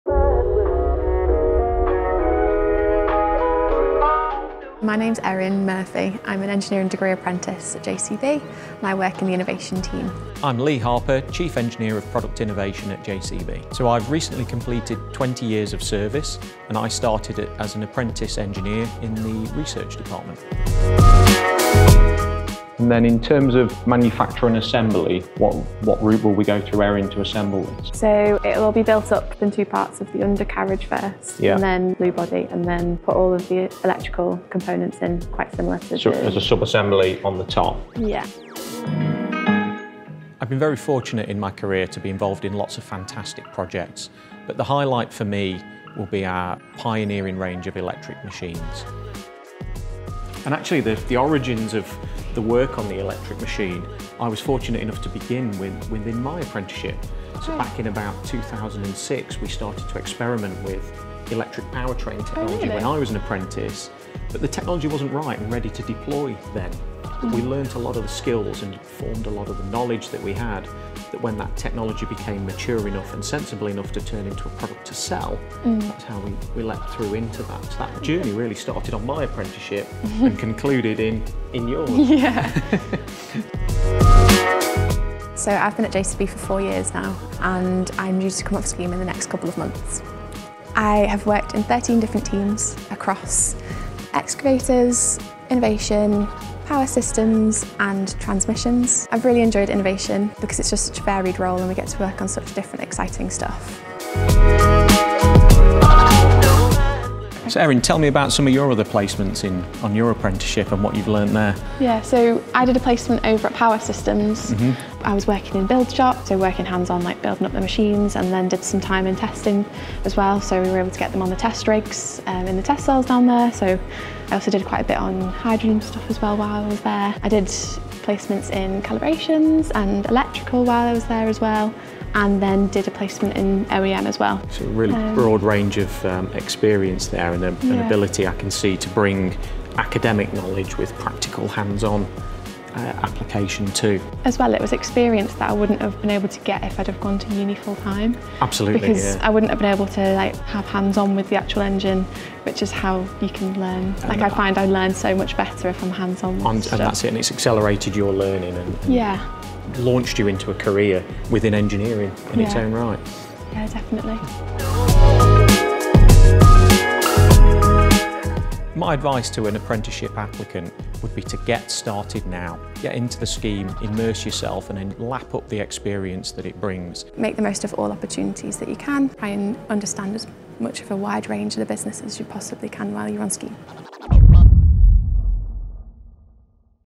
Past vs present: Apprenticeships at JCB
Apprenticeships past and present. Erin, a current Engineering Degree Apprentice, talks with Lee, Chief Engineer and former apprentice, about their experiences on JCB’s Early Careers schemes.